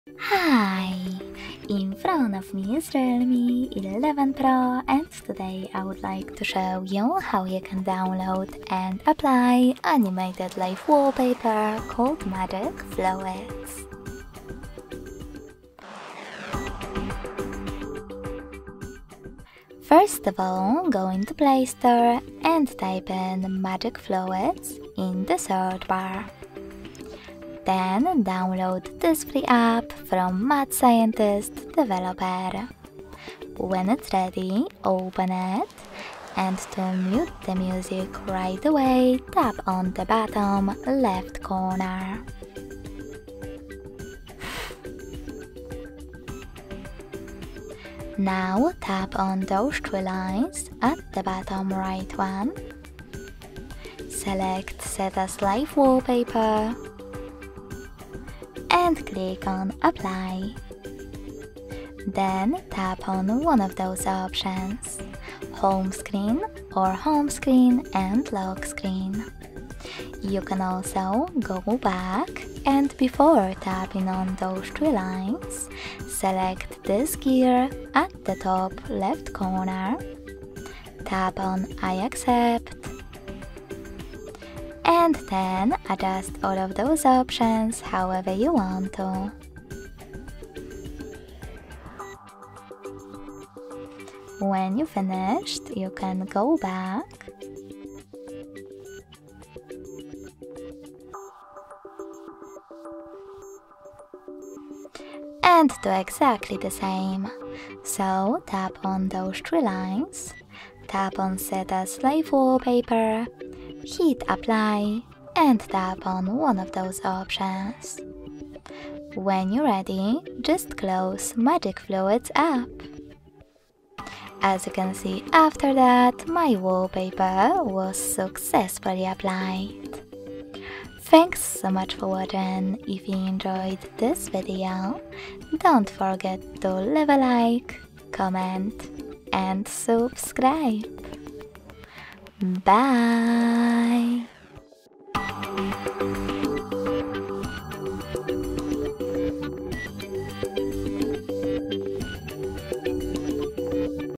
How to Get Live Wallpaper on Realme 11 Pro - Animated Wallpaper
In this informative tutorial video, we'll guide you through the process of setting up a live wallpaper on your Realme 11 Pro using the Magic Fluids app. Discover the enchanting world of animated wallpapers and learn how to bring your device's home screen to life with mesmerizing fluid simulations, all for free!

What are the steps to set up an animated wallpaper on my Realme 11 Pro?